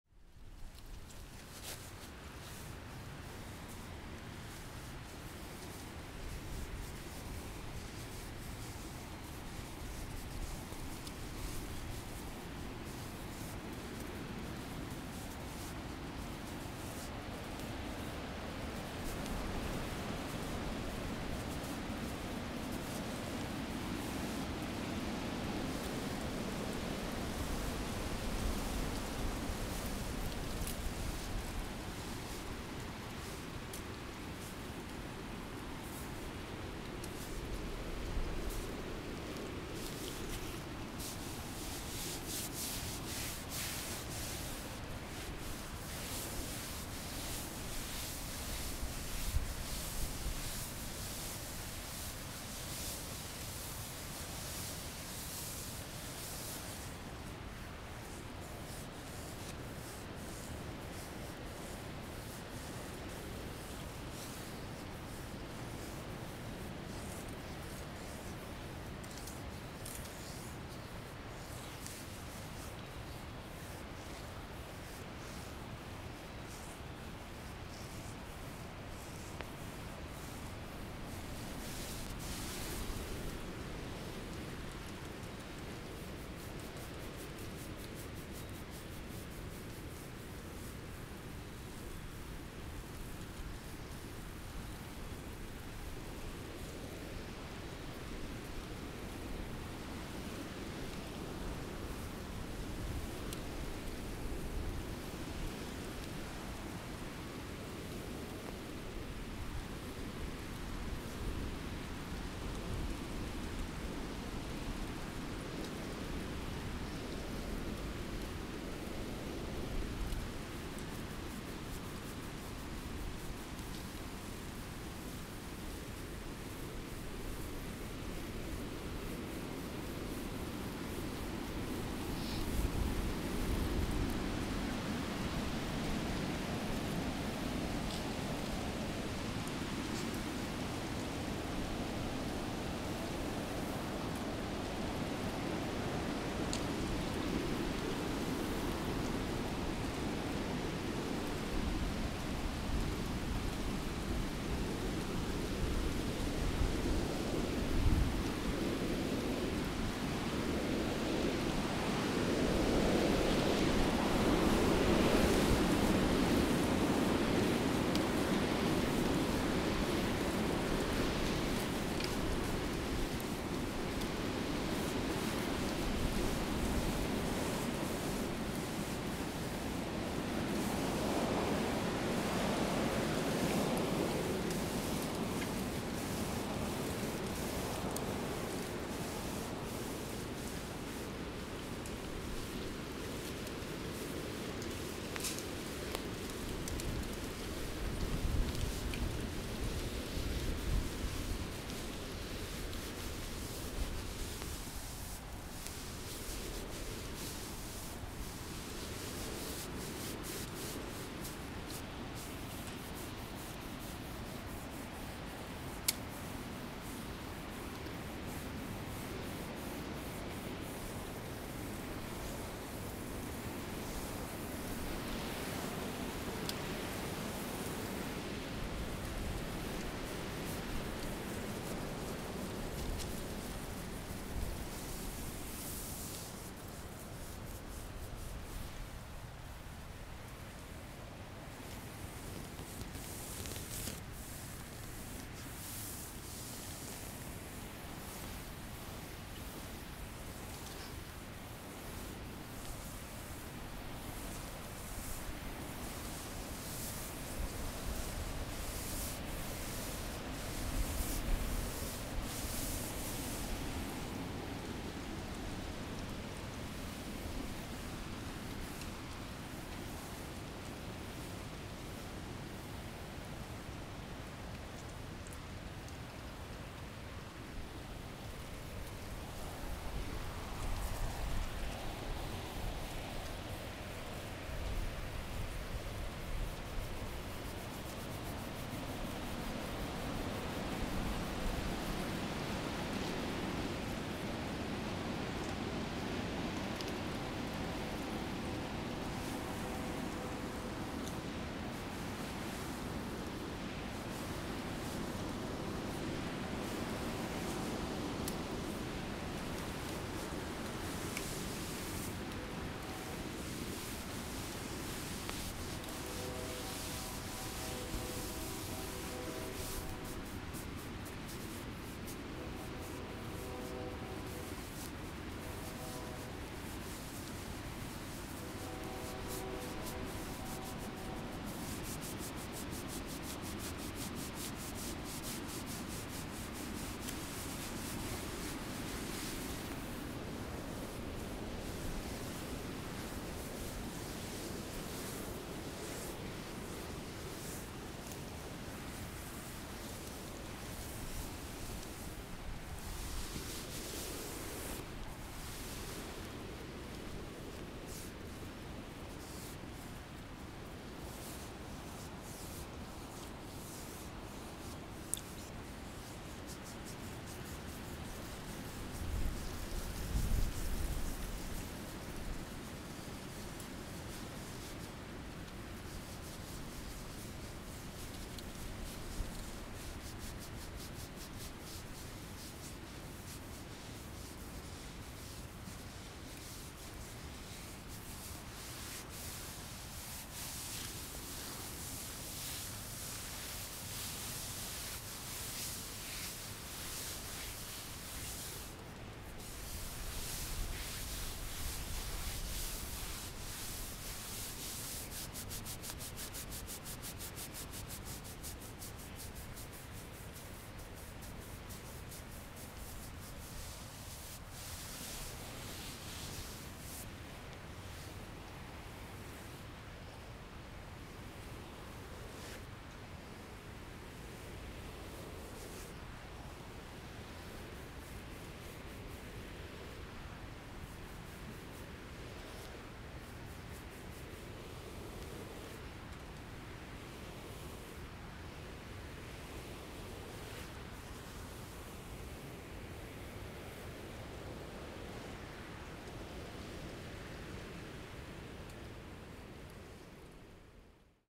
Outdoor ASMR ☀️ Arm & Shoulder Massage~Scalp Massage~No Talking
The weather was too nice not to be outside so I hope you all enjoy this ASMR massage by the river 😴

Relax with the soothing rustle of wind through the trees, the continuous, peaceful rushing of a river, and the occasional distant rumble of a train passing by.